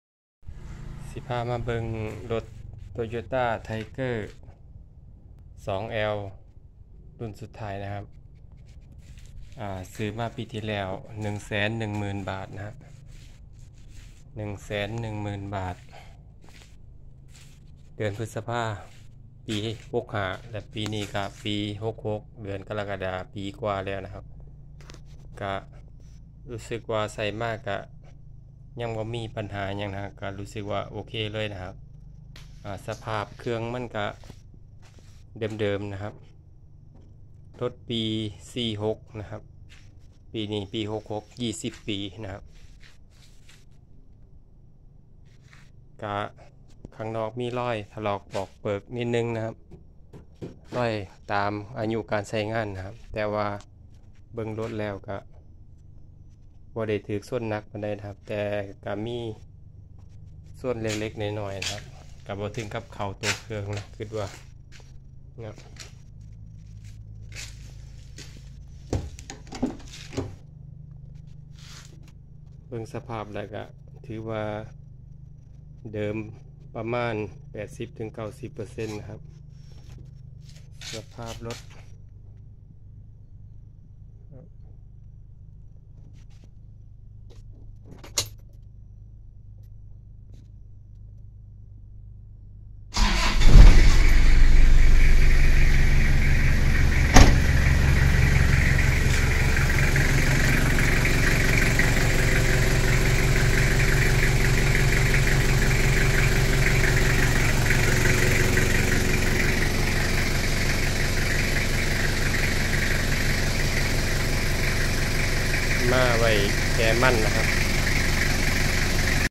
กระบะ#โตโยต้า#ไทเกอร์ 2 L ตอนเดียว สภาพเดิมๆ พอใช้ ใช้งานคุ้มดี เข้าไร่เข้าสวน ราคา 110,000 บาท
รถมือสองน่าใช้ รถกระบะเข้าไร่เข้าสวน กระบะตอนเดียว เครื่องไมตี้เอ็ก